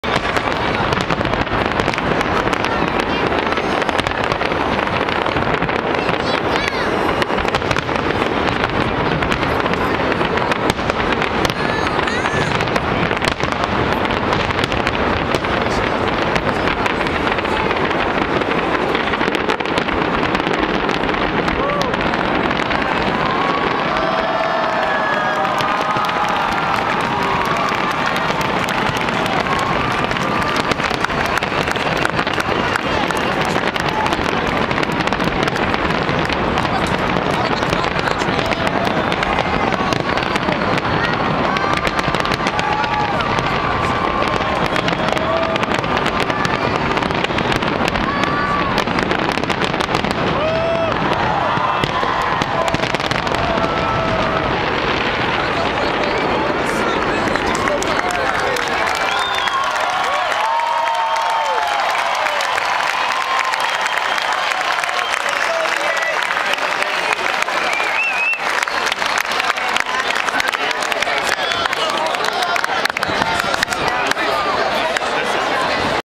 090704 Macy's Fireworks Hudson River 42nd Street
short video of the macy's 4th of july fireworks spectacular held on the hudson river in celebration of the 400th anniversary of henry hudson's sail up the river. in the video you can see almost two of 6 barges shooting off identical versions up the length of the river. imagine this crowd on every street up and down manhattan. i'm on 42nd street. when the camera turns at the end you can see times square at the end of the street.